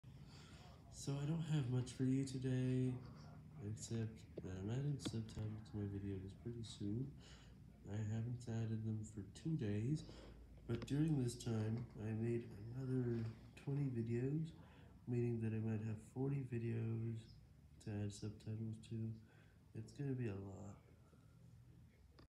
I will be adding subtitles to my videos soon.
I was telling you that subtitles will be added to my videos, but the last time I did that was about 2 days ago.